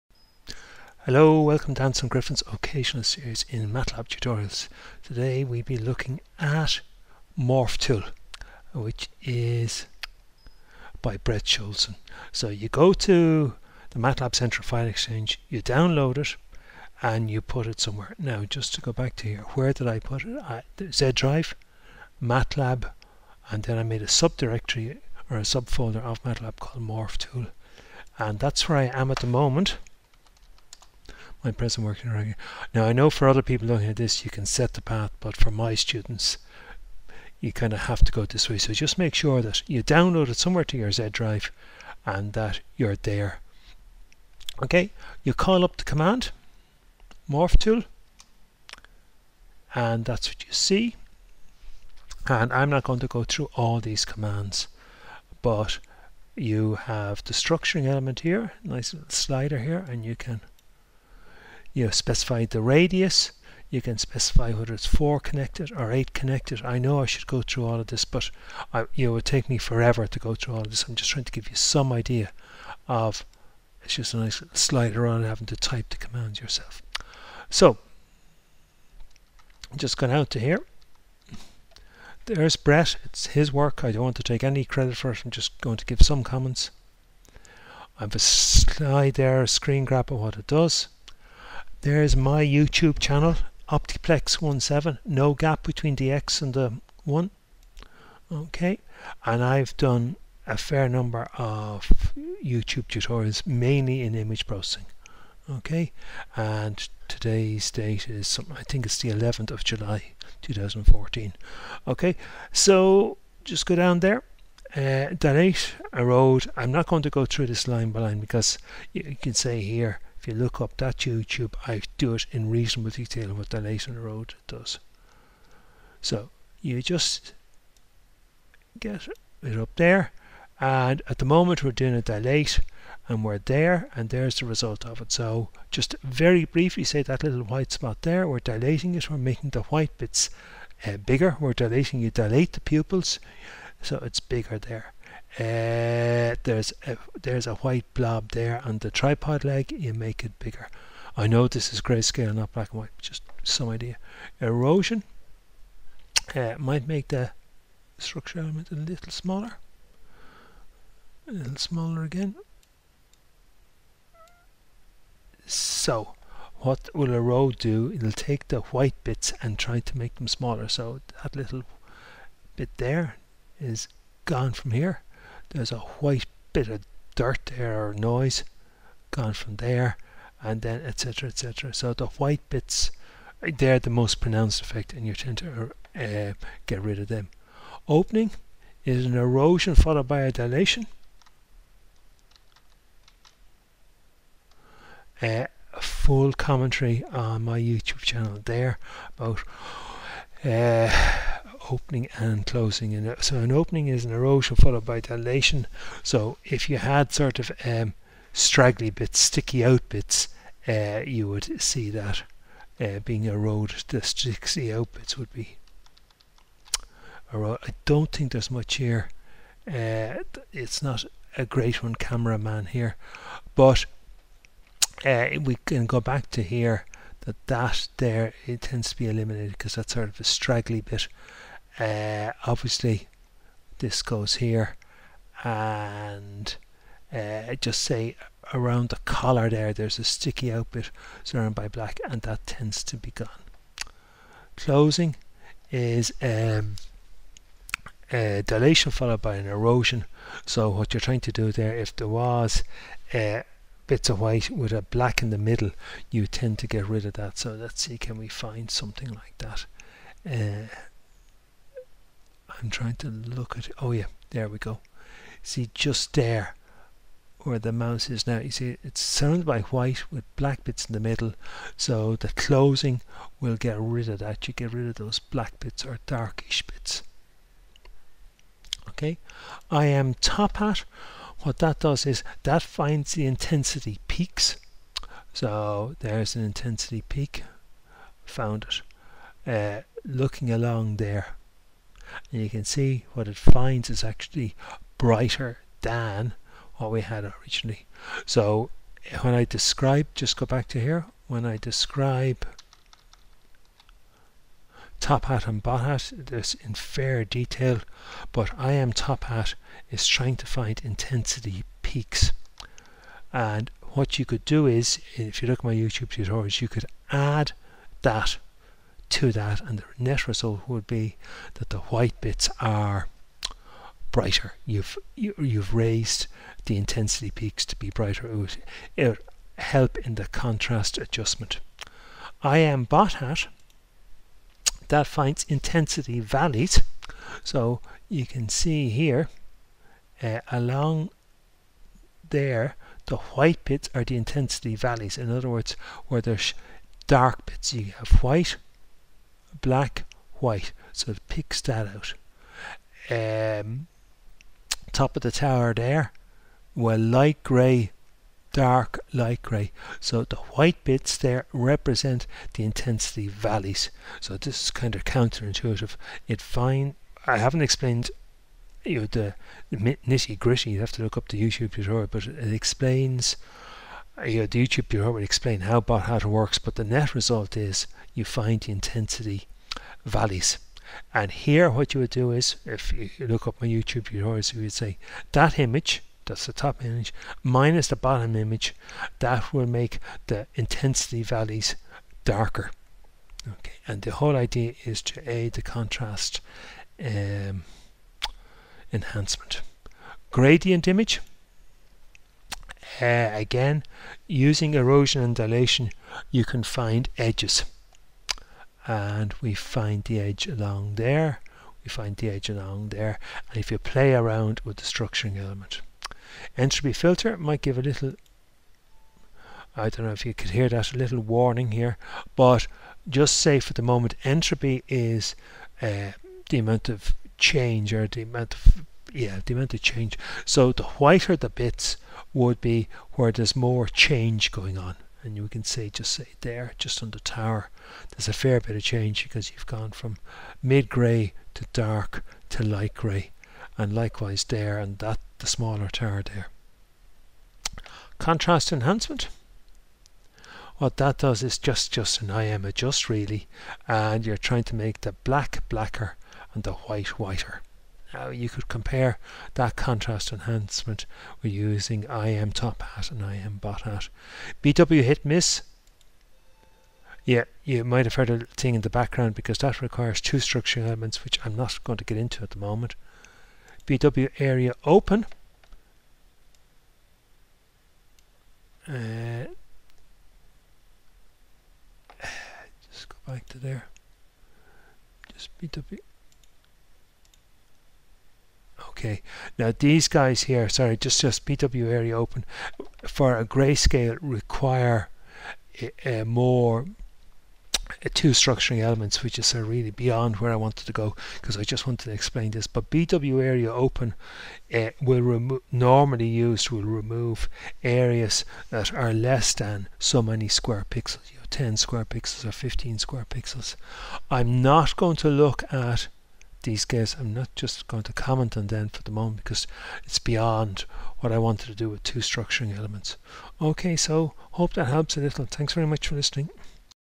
MorphTool Matlab demonstrates bwmorph, bwdist, imtophat, imbothat and bwareaopen
A commentary of Brett Shoelson's Morph Tool